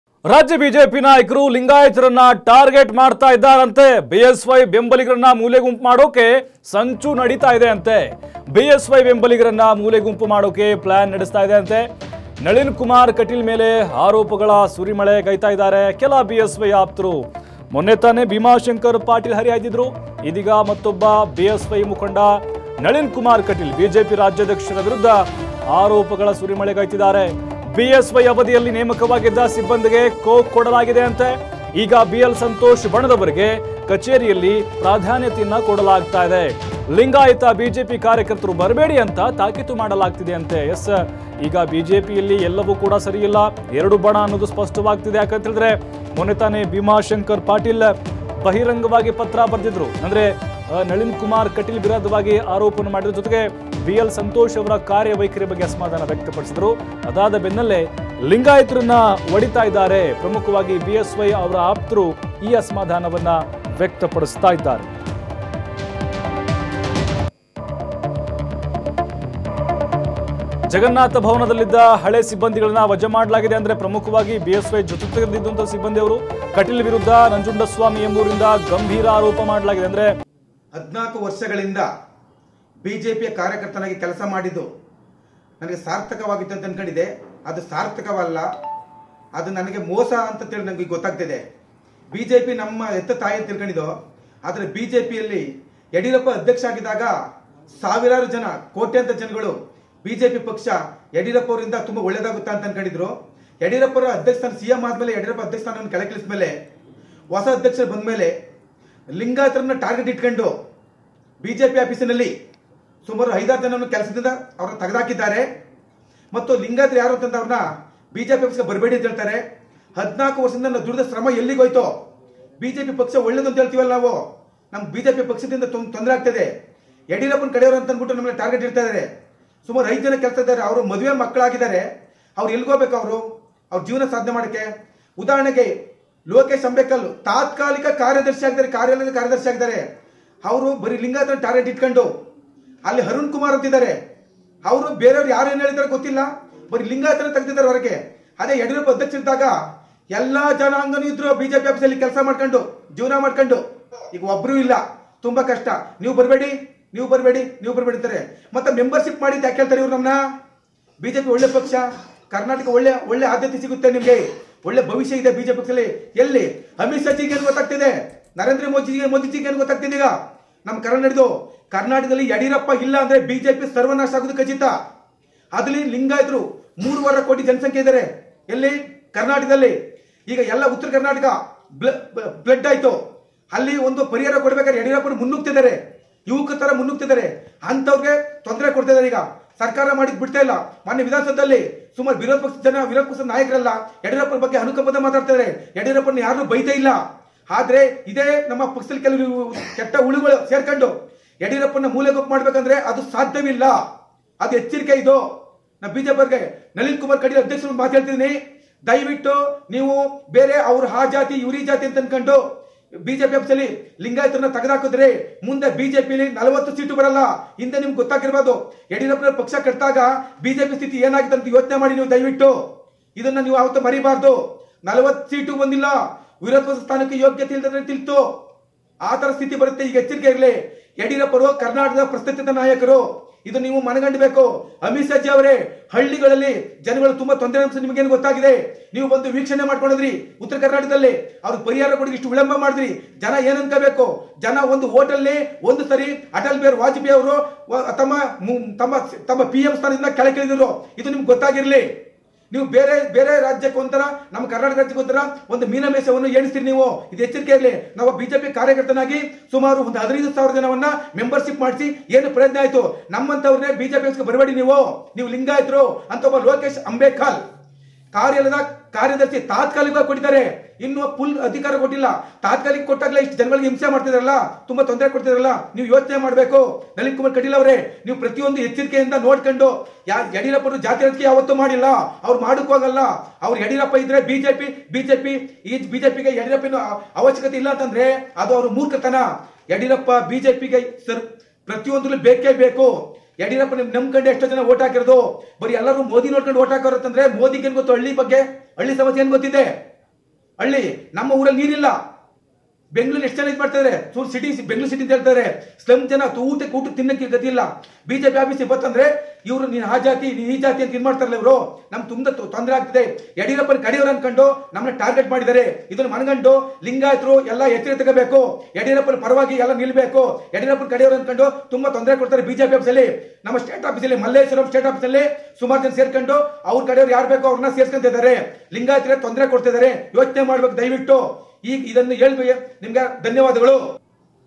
BSY ಬೆಂಬಲಿಗರನ್ನು ಮೂಲೆಗುಂಪು ಮಾಡಲು ಪ್ಲ್ಯಾನ್? ಬಿ.ಎಲ್ ಸಂತೋಷ್ ಬಣದವರಿಗೆ ಕಚೇರಿಯಲ್ಲಿ ಪ್ರಾಧಾನ್ಯತೆ..!?
ಬಿ.ಎಸ್.ವೈ ಬೆಂಬಲಿಗರನ್ನು ಮೂಲೆಗುಂಪು ಮಾಡಲು ಪ್ಲ್ಯಾನ್?- ಬಿ.ಎಲ್ ಸಂತೋಷ್ ಬಣದವರಿಗೆ ಕಚೇರಿಯಲ್ಲಿ ಪ್ರಾಧಾನ್ಯತೆ- ಬಿ.ಎಸ್‌.ವೈ ಅವಧಿಯಲ್ಲಿ ನೇಮಕವಾಗಿದ್ದ ಸಿಬ್ಬಂದಿಗೆ ಕೊಕ್ ಆರೋಪ